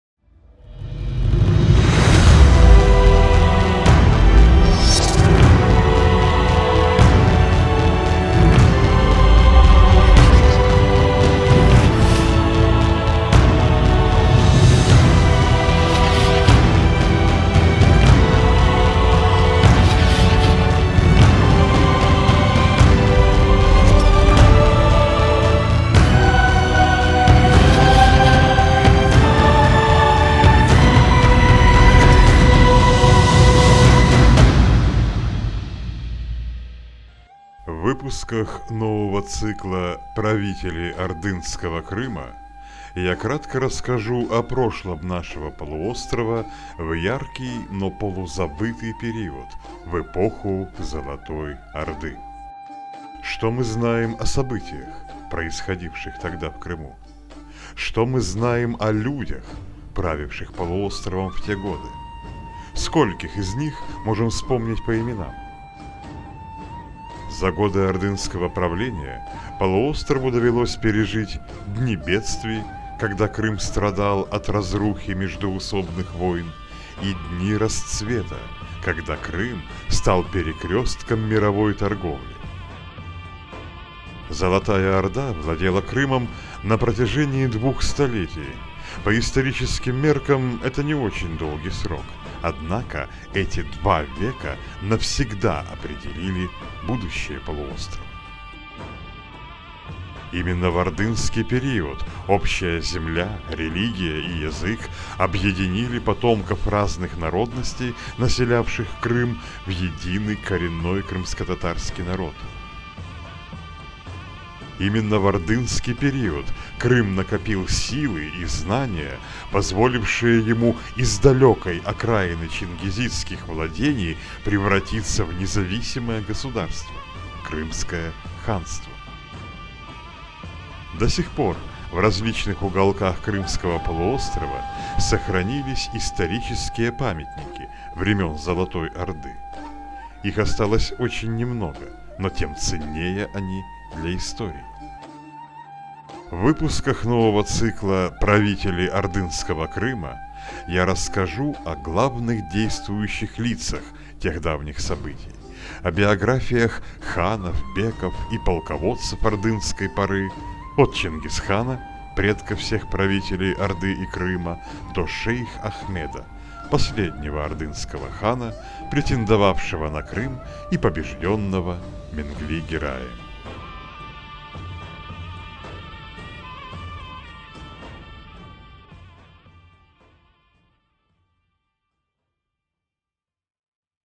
Правители ордынского Крыма. Выпуск 1. Крым в эпоху Золотой Орды
Представляем первый выпуск нового цикла «Ордынские правители Крыма». Эпоха Золотой Орды - один из самых малоизученных периодов в истории Крыма. Что мы знаем об исторических деятелях, правивших нашим полуостровом на протяжении двухсот лет до возникновения независимого Крымского ханства? Олекса Гайворонский кратко расскажет о наиболее выдающихся личностях той эпохи.